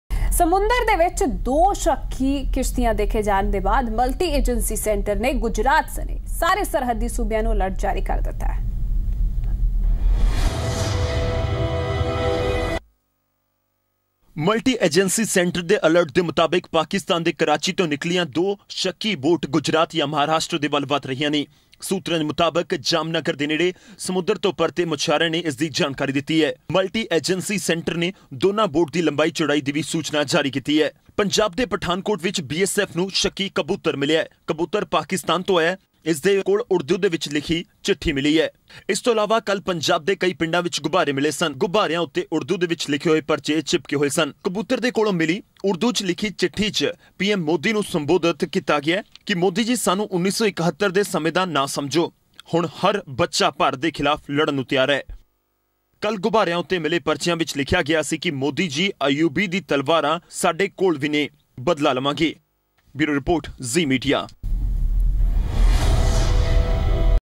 Multi center agency issued alert in all the border states
Multi center agency issued alert in all the border states. Watch this special segment and get to know more here.